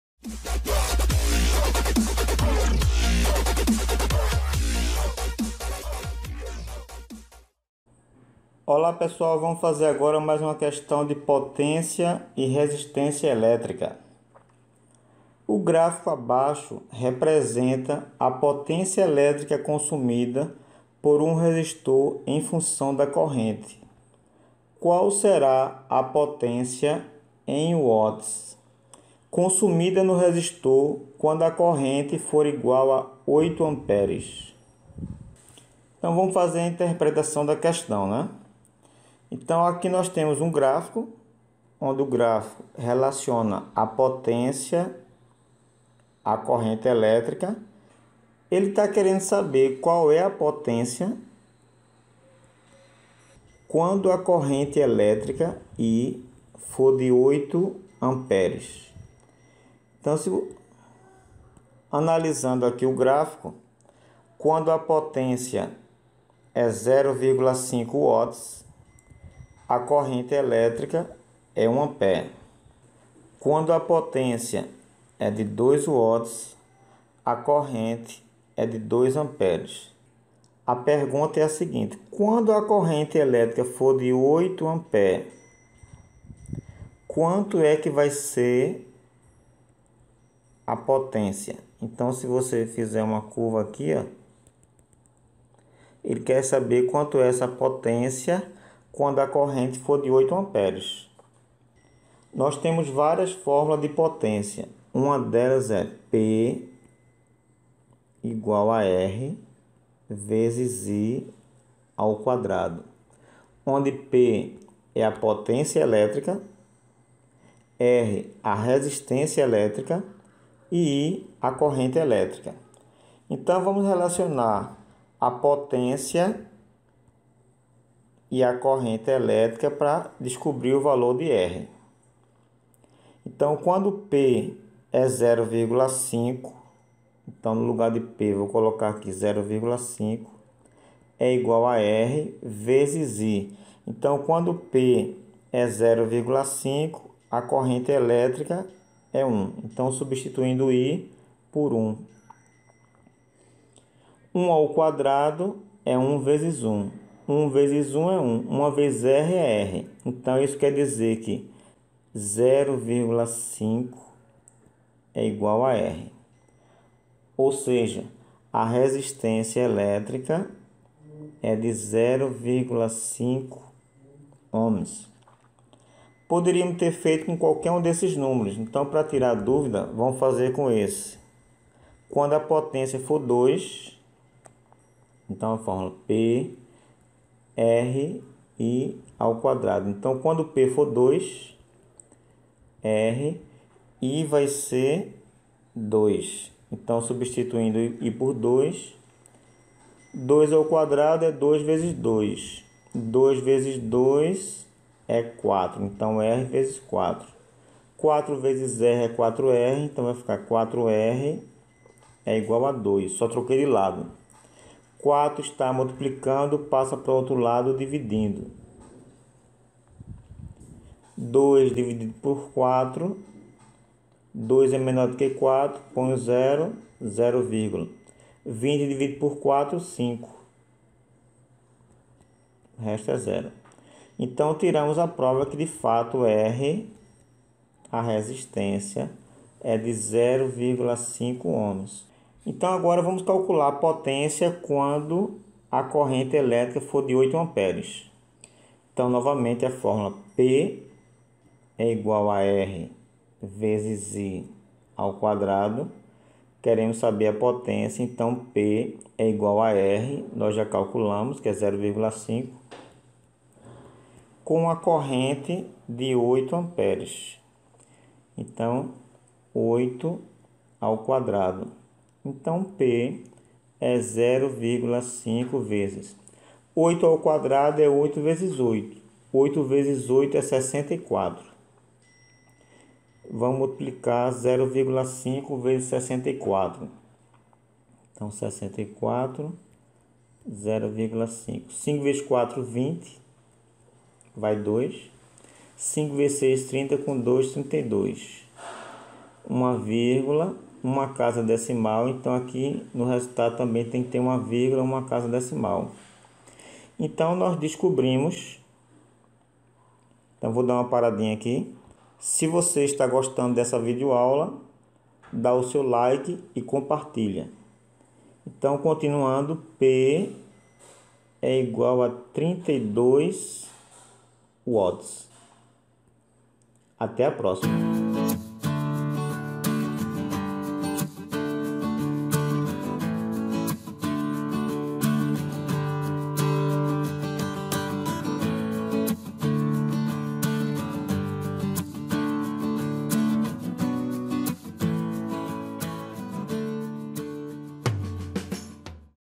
Física - Potência e Resistência Elétrica - Q3 (Digital)
Aula de Física - Potência e Resistência Elétrica - Q3 - Prof. Edson Dias.

Exercício resolvido de Potência e Resistência Elétrica.
Se gostar clique aqui em cima em "gostei" , "compartilhar" e "favorito".